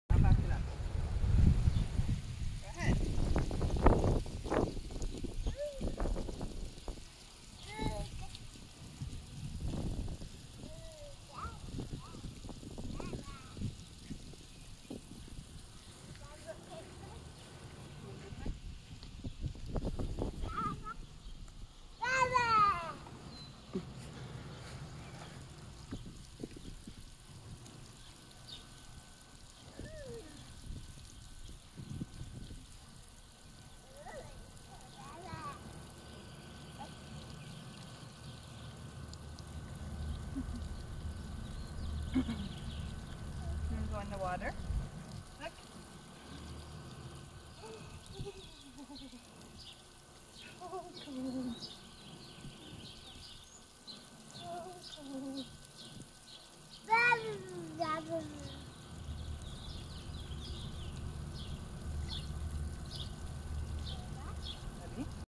Fun in the sun!
Enjoying the sun, playing in my pool